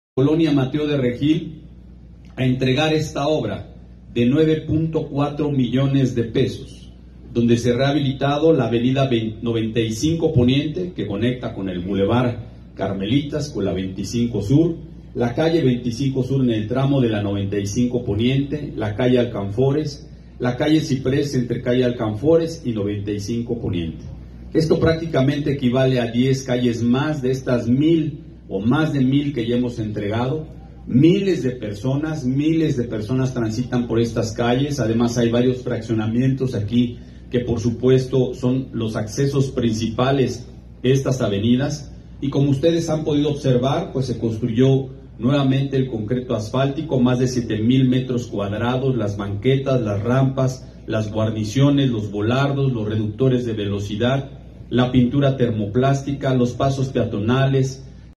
El Ayuntamiento de Puebla entregó la rehabilitación de las calles 95 Poniente, 25 Sur y Ciprés
El Ayuntamiento de Puebla entregó la rehabilitación de las calles 95 Poniente, 25 Sur y Ciprés, ubicadas en la colonia Mateo de Regil. El alcalde Eduardo Rivera Pérez indicó que su administración invirtió 9.4 millones de pesos para estos trabajos.